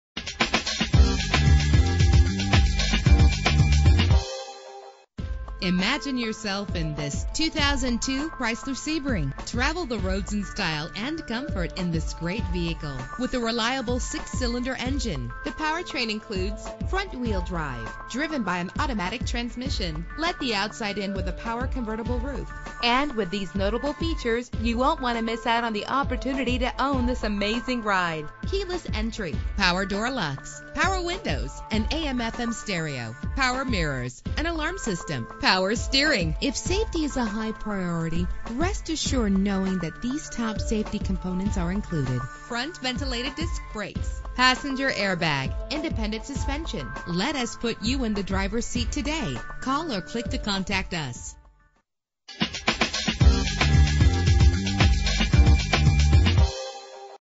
2002 Chrysler Sebring - Lakeland FL
Year: 2002
Make: Chrysler
Model: Sebring
Trim: LX
Engine: 2.7 liter 6 cylinder 24 valve
Transmission: 4-Speed Automatic
Color: Green
Mileage: 152811
Address:
5210 S. Florida Ave.